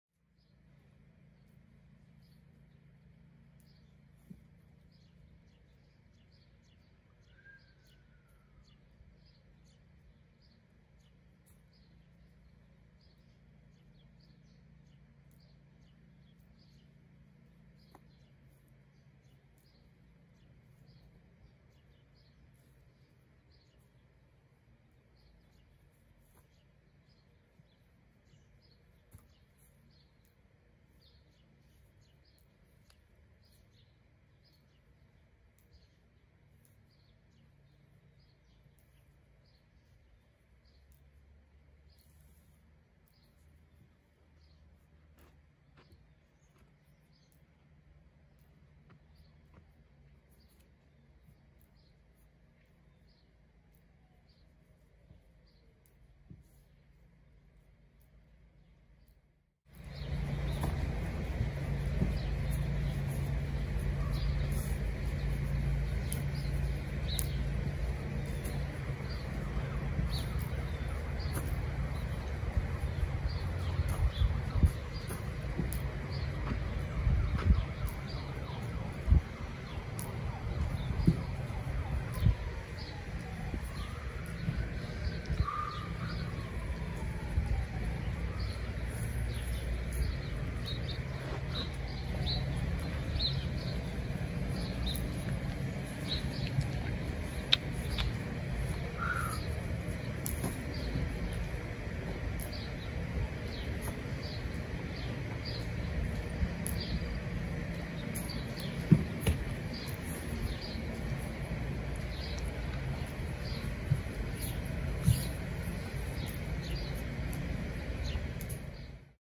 Alfie Eating His Hay
A horse named Alfie eating his hay.